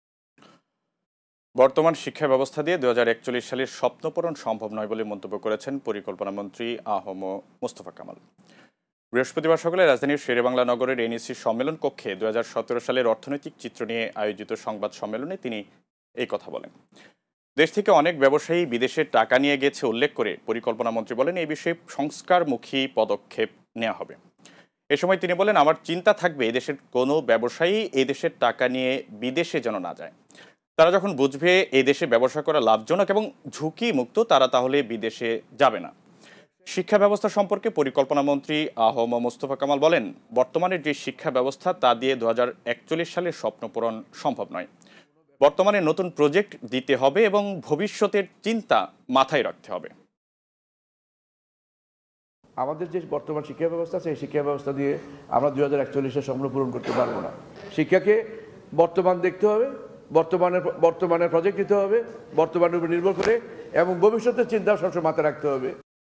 বর্তমান শিক্ষাব্যবস্থা দিয়ে ২০৪১ সালের স্বপ্ন পূরণ সম্ভব নয় -আ হ ম মুস্তফা কামাল
বর্তমান শিক্ষাব্যবস্থা দিয়ে ২০৪১ সালের স্বপ্ন পূরণ সম্ভব নয় বলে মন্তব্য করেছেন পরিকল্পনামন্ত্রী আ হ ম মুস্তফা কামাল। বৃহস্পতিবার সকালে রাজধানীর শেরেবাংলা নগরের এনইসি সম্মেলন কক্ষে ২০১৭ সালের অর্থনৈতিক চিত্র নিয়ে আয়োজিত সংবাদ সম্মেলনে এ কথা বলেন তিনি।

দেশ থেকে অনেক ব্যবসায়ী বিদেশে টাকা নিয়ে গেছে উল্লেখ করে পরিকল্পনামন্ত্রী বলেন, এ বিষয়ে সংস্কারমূখী পদক্ষেপ নেয়া হবে। এ সময় তিনি বলেন, 'আমার চিন্তা থাকবে এ দেশের কোনো ব্যবসায়ী এ দেশের টাকা নিয়ে বিদেশে যেনো না যায়। তারা যখন বুঝবে এ দেশে ব্যবসা করা লাভজনক এবং ঝুঁকিমুক্ত তারা তাহলে বিদেশে যাবে না।

শিক্ষা ব্যবস্থা সর্ম্পকে পরিকল্পনামন্ত্রী আ হ ম মুস্তফা কামাল বলেন,বর্তমানের যে শিক্ষা ব্যবস্থা তা দিয়ে ২০৪১ সালের স্বপ্নপূরণ সম্ভব নয়। বর্তমানে নতুন প্রজেক্ট দিতে হবে এবং ভবিষ্যৎ এর চিন্তা মাথায় রাখতে হবে।